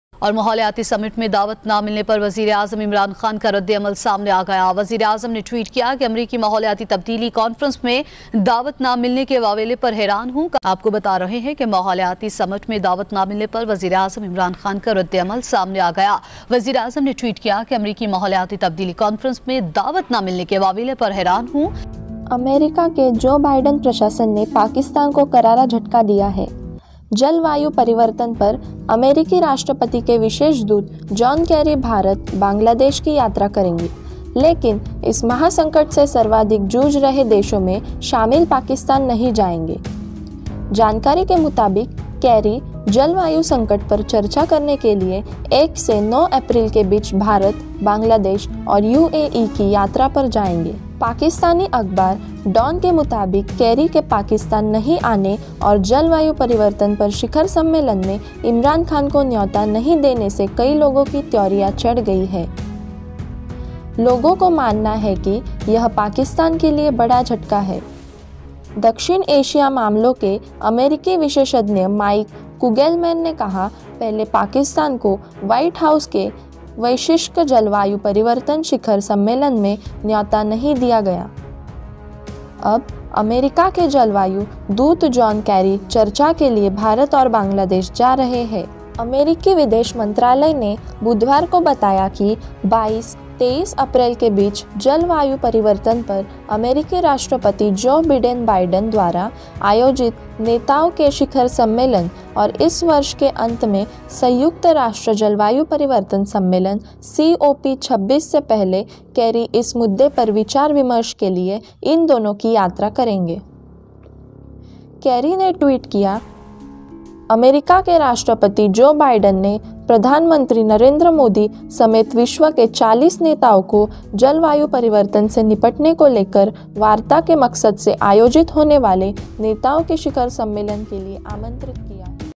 Shikhar Sammelan me Pakistan PM Imran khan ko nyota nahi | Modi ko special invitation
अमेरिका के जो बाइडेन प्रशासन ने पाकिस्‍तान को करारा झटका दिया है। जलवायु परिवर्तन पर अमेरिकी राष्‍ट्रपति के विशेष दूत जॉन कैरी भारत, बांग्‍लादेश की यात्रा करेंगे लेकिन इस महासंकट से सर्वाधिक जूझ रहे देशों में शामिल पाकिस्‍तान नहीं जाएंगे। जानकारी के मुताबिक कैरी जलवायु संकट पर चर्चा करने के लिए एक से नौ अप्रैल के बीच भारत, बांग्लादेश और यूएई की यात्रा पर जाएंगे।

पाकिस्‍तानी अखबार डॉन के मुताबिक केरी के पाकिस्‍तान नहीं आने और जलवायु परिवर्तन पर शिखर सम्‍मेलन में इमरान खान को न्‍योता नहीं देने से कई लोगों की त्योरियां चढ़ गई हैं। लोगों को मानना है कि यह पाकिस्‍तान के लिए बड़ा झटका है। दक्षिण एशियाई मामलों के अमेरिकी विशेषज्ञ माइक कुगेलमैन ने कहा, 'पहले पाकिस्‍तान को वाइट हाउस के वैश्विक जलवायु परिवर्तन शिखर सम्‍मेलन में न्‍यौता नहीं दिया गया। अब अमेरिका के जलवायु दूत जॉन कैरी चर्चा के लिए भारत और बांग्‍लादेश जा रहे हैं।'

अमेरिकी विदेश मंत्रालय ने बुधवार को बताया कि 22-23 अप्रैल के बीच जलवायु परिवर्तन पर अमेरिकी राष्ट्रपति जो बिडेन बाइडन द्वारा आयोजित ‘नेताओं के शिखर सम्मेलन’ और इस वर्ष के अंत में संयुक्त राष्ट्र जलवायु परिवर्तन सम्मेलन (सीओपी26) से पहले कैरी इस मुद्दे पर विचार विमर्श के लिए इन देशों की यात्रा करेंगे। कैरी ने ट्वीट किया, ‘जलवायु संकट से निपटने के लिए अमीरात, भारत और बांग्लादेश में दोस्तों के साथ सार्थक चर्चा को लेकर उत्साहित हूं।’

40 नेताओं संग जलवायु परिवर्तन से निपटने को लेकर वार्ता
अमेरिका के राष्ट्रपति जो बाइडन ने प्रधानमंत्री नरेन्द्र मोदी समेत विश्व के 40 नेताओं को जलवायु परिवर्तन से निपटने को लेकर वार्ता के मकसद से आयोजित होने वाले ‘नेताओं के शिखर सम्मेलन’ के लिए आमंत्रित किया है

वाइट व्हाइट हाउस के अनुसार, प्रधानमंत्री मोदी के अलावा चीन के राष्ट्रपति शी जिनपिंग, रूस के राष्ट्रपति व्लादिमीर पुतिन, जापान के प्रधानमंत्री योशिहिदे सुगा, ब्राजील के राष्ट्रपति जायर बोलसोनारो, कनाडा के प्रधानमंत्री जस्टिन ट्रूडो, इजराइल के प्रधानमंत्री बेंजामिन नेतन्याहू, सऊदी अरब के शाह सलमान बिन अब्दुलअजीज अल सऊद और ब्रिटेन के प्रधानमंत्री बोरिस जॉनसन समेत 40 नेताओं को शिखर सम्मेलन के लिए आमंत्रित किया गया है। इस कार्यक्रम का सीधा प्रसारण किया जाएगा।

इन नेताओं के अलावा दक्षिण एशिया से बंग्लादेश की प्रधानमंत्री शेख हसीना और भूटान के प्रधानमंत्री लोते शेरिंग को भी सम्मेलन में शामिल होने के लिए आमंत्रित किया गया है। वाइट हाउस ने कहा कि इस शिखर सम्मेलन और सीओपी26 का मुख्य लक्ष्य वैश्विक तापमान को 1.5 डिग्री सेल्सियस से नीचे रखने के प्रयासों को गति देना है। उसने कहा कि इस सम्मेलन में इन उदाहरणों को भी रेखांकित किया जाएगा कि जलवायु महत्वाकांक्षा से अच्छे वेतन वाली नौकरियां कैसे पैदा होती हैं,